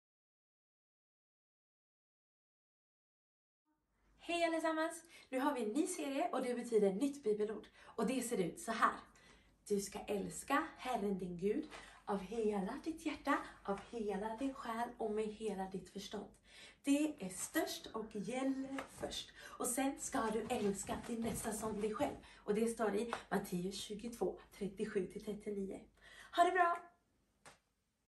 Vision24 - Bibelord
Under denna serie tränar vi på bibelordet från Matteusevangeliet kapitel 22, vers 37-39. Häng på oss du med!